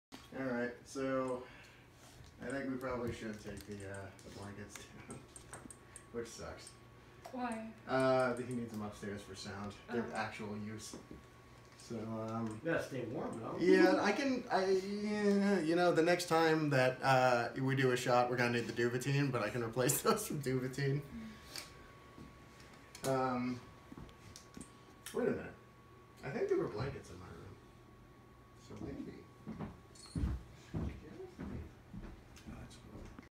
ONE - The Movie - Video Diary - 10/15/2016
ONE - The Movie stars Lance Henriksen - The final scenes are shooting right now.